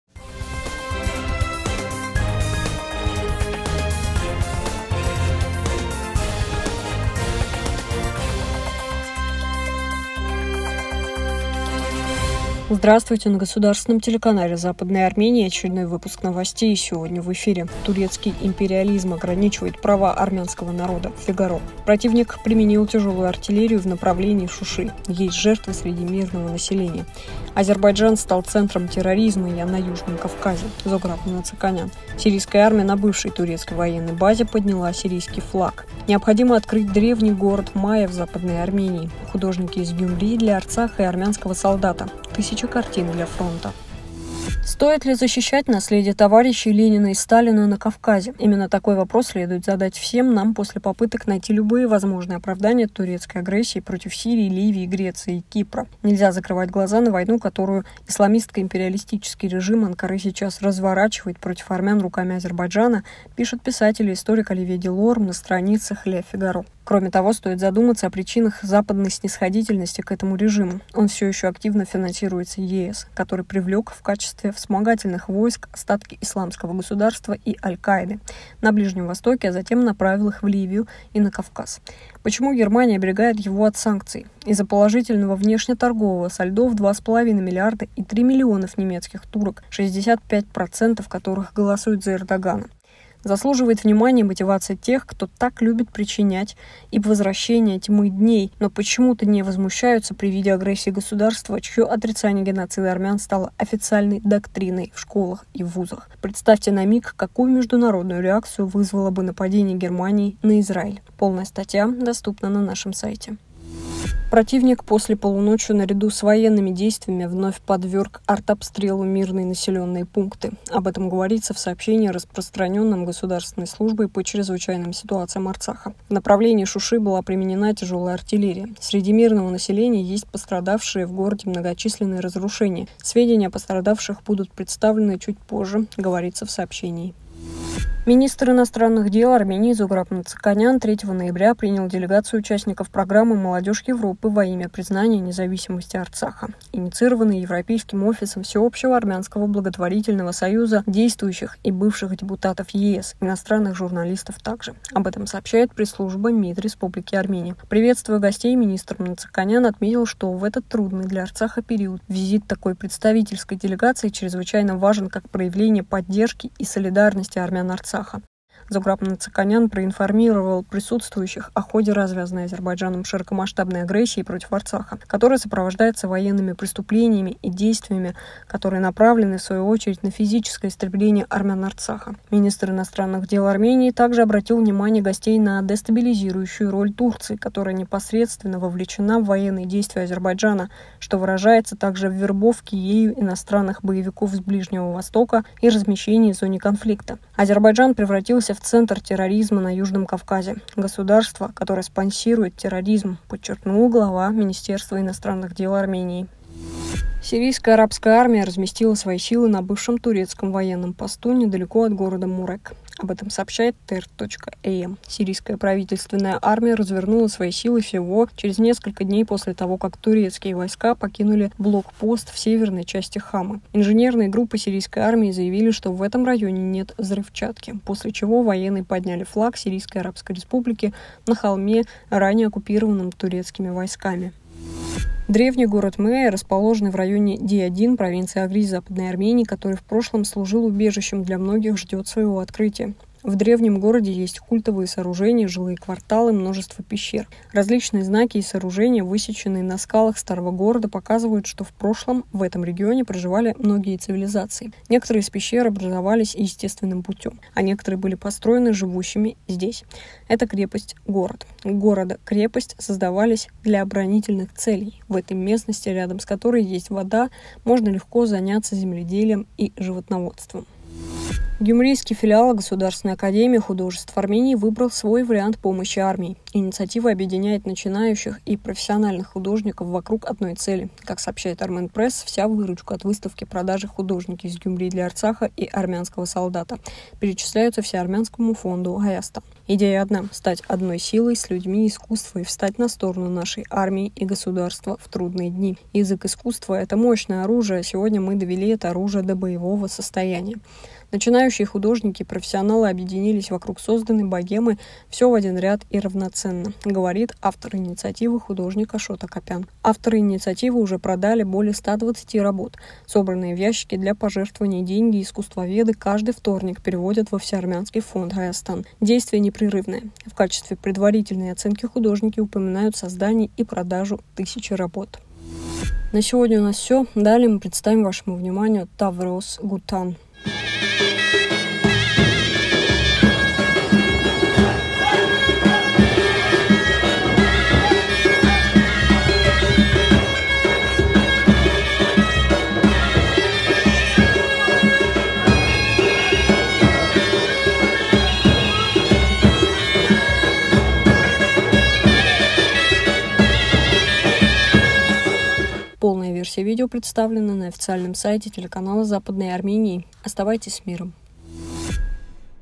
Азербайджан стал центром терроризма на Южном Кавказе.Новости 04.11.2020
1. «Турецкий империализм ограничивает права армянского народа» ․ Фигаро:
2. Противник применил тяжелую артиллерию в направлении Шуши. Есть жертвы среди мирного населения
3. Азербайджан стал центром терроризма на Южном Кавказе. Зограб Мнацаканян
4. Сирийская армия на бывшей турецкой военной базе подняла сирийский флаг .
5. Необходимо открыть древний город Мейя в Западной Армении.
6. «Художники из Гюмри для Арцаха и армянского солдата»: тысяча картин для фронта